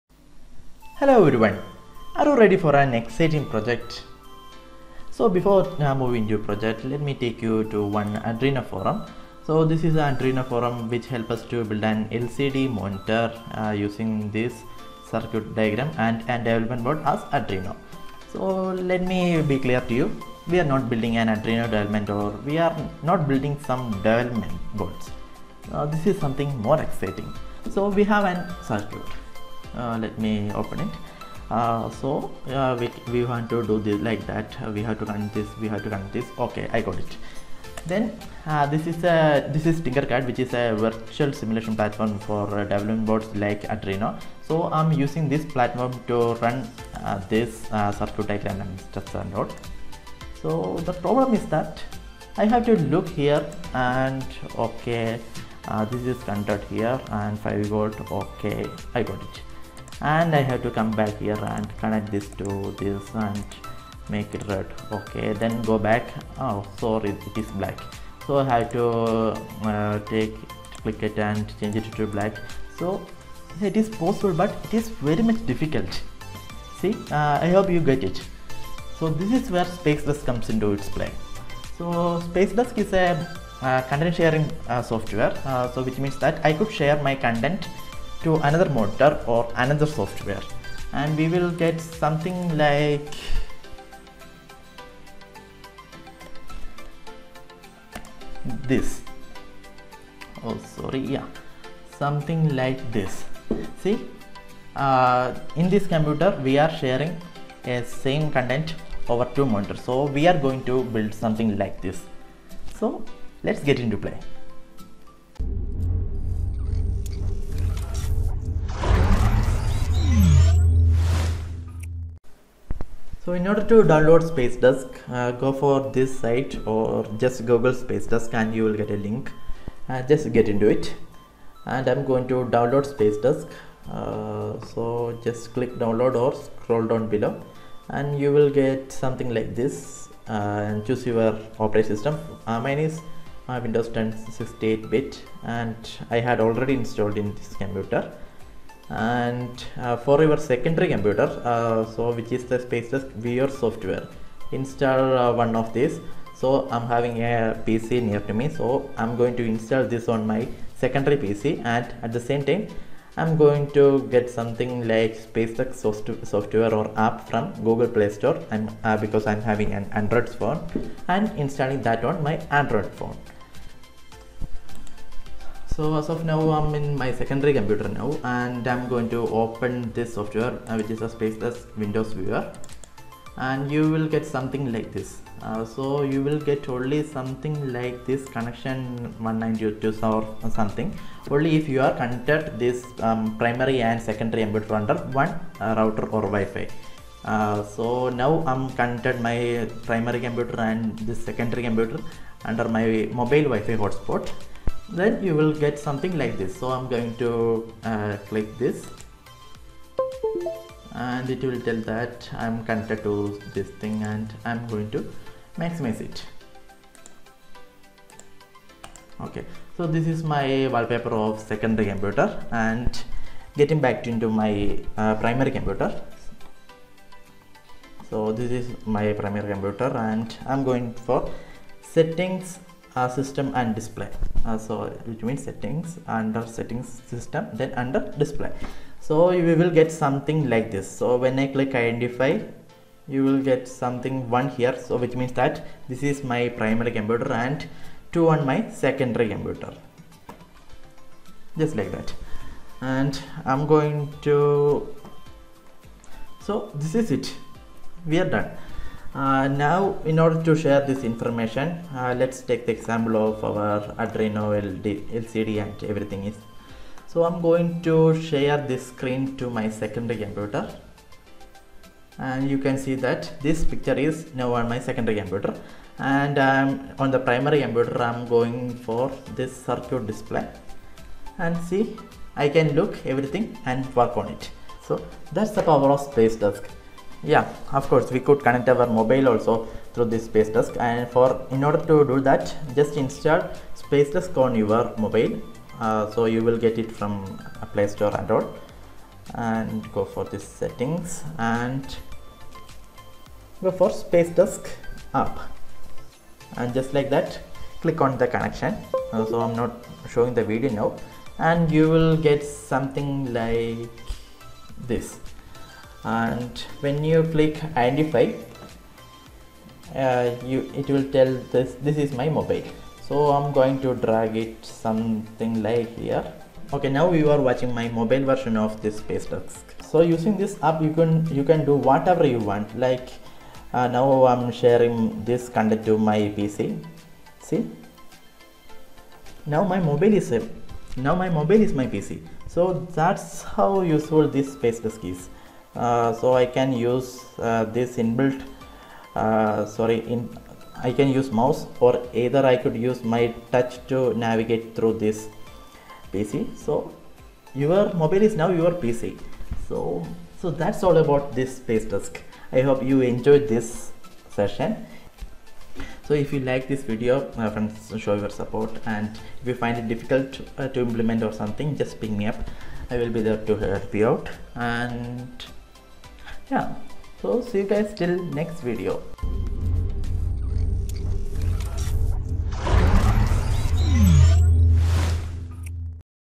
SpaceDesk | Multi-Monitor without Cables | Productivity Hack for Computer Enthusiasts and Gamers
Hello Viewers,

Today I'm introducing you to a Software called Spacedesk through which you could share your PC screen without wires or cable but over Wi-Fi.
You could even share it to your mobile using the SpaceDesk App. In video tells/helps you how to install it and use it for your Convenience.

Hope you Enjoy the Video, Comment your Feedback